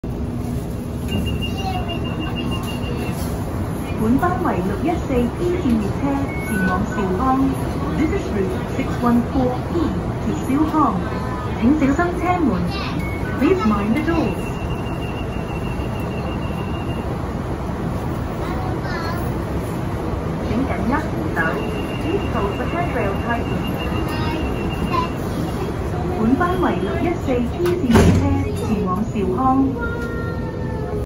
港鐵輕鐵（路綫614P）第一期列車（1044）開關門片段
車隊編號：1044
拍攝地點：鳳地站
行走路綫：614P
目的地：兆康